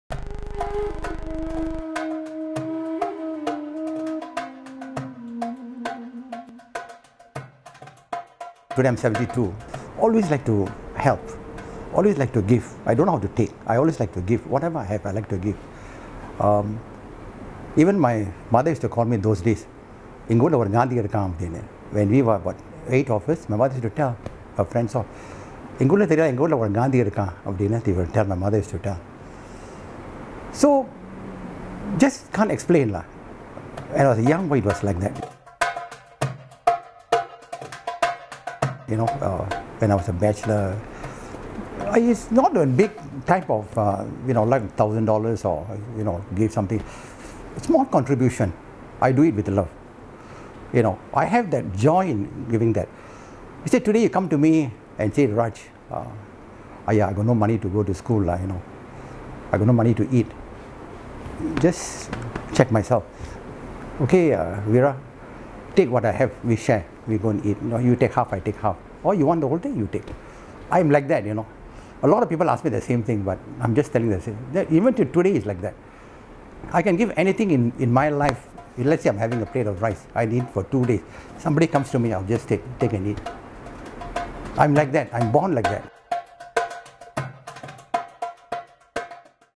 Paul1of6_GANDHI IN THE HOUSE
From a young age Paul Raj had an innate desire to reach out to fellow humans. Even in hardship, when he had nothing and slept on the floor without mattress, he would give what ever he had. The desire to help was deep rooted.